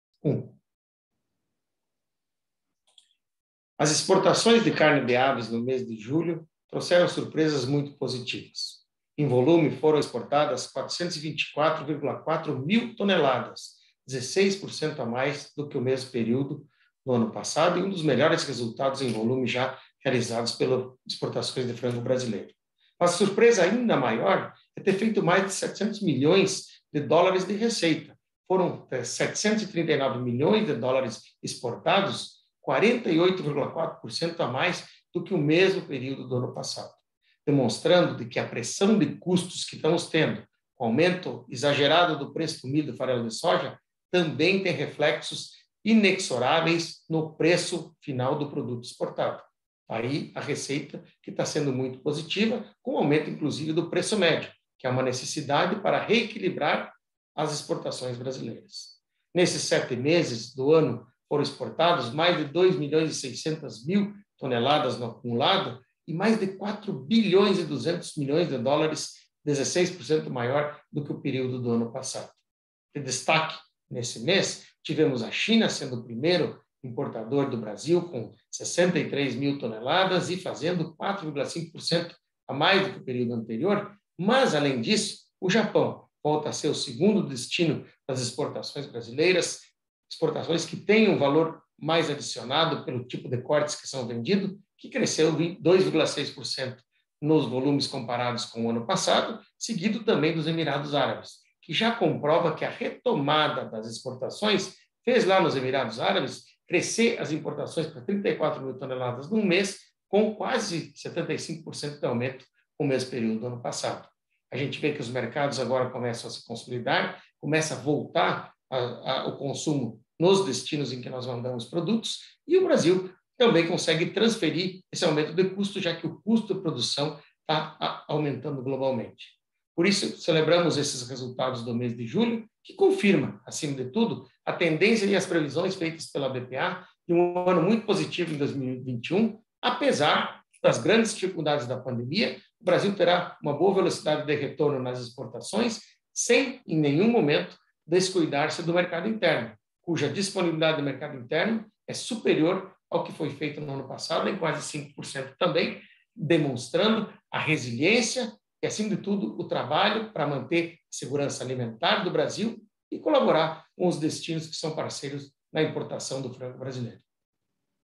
Exportações de carne de frango crescem 16,4% em julho
A Associação Brasileira de Proteína Animal (ABPA) informa que as exportações brasileiras de carne de frango (considerando in natura e processados) totalizaram 424,4 mil toneladas em julho. O número é 16,4% superior ao alcançado no mesmo período de 2020, quando foram exportadas 364,6 mil toneladas. É o melhor resultado mensal de 2021 e o terceiro maior da história do setor produtivo.

Em receita, os embarques totalizaram US$ 739,2 milhões, número 48,4% superior ao alcançado em julho do ano passado — com US$ 498,2 milhões. O último registro de receita mensal de exportações do setor acima de US$ 700 milhões tinha acontecido em julho de 2018.

“Com mercados de alto valor agregado ocupando os primeiros postos entre os principais destinos, houve uma forte elevação no resultado final das vendas de julho, com impacto direto no saldo do ano.  Neste contexto, houve o natural repasse dos custos de produção, que tem impactado os preços da proteína animal não apenas do Brasil, mas também no mercado internacional”, avalia o presidente da ABPA, Ricardo Santin.

No acumulado do ano, as vendas de carne de frango alcançaram 2,668 milhões de toneladas. O resultado é 7,98% maior do que o registrado no período de janeiro a julho de 2020, quando foram embarcadas 2,471 milhões de toneladas. Em receita, o resultado acumulado nos sete primeiros meses deste ano totalizou US$ 4,216 bilhões. O valor significa um aumento de 15,7% na comparação com o mesmo período de 2020 — com US$ 3,642 bilhões.

Entre os principais mercados importadores, o destaque foi a China, com 63,1 mil toneladas exportadas em julho — 4,5% a mais do que o exportado no mesmo período de 2020.  Assumindo o segundo lugar, o Japão importou 35,7 mil toneladas (+2,6%). Em terceiro lugar e destaque principal no mercado halal, os Emirados Árabes Unidos importaram 34 mil toneladas em julho (+75,7%). A África do Sul aparece em quarto lugar com 24,6 mil toneladas (+36%) importadas do Brasil.

“A recuperação econômica de diversos países importadores e o aumento substancial no número de pessoas vacinadas contra a Covid-19 no mundo têm colaborado para os volumes alcançados nos últimos meses. A tendência é de volumes altos nas exportações até o final do ano, neste contexto", avalia o diretor de mercados, Luis Rua.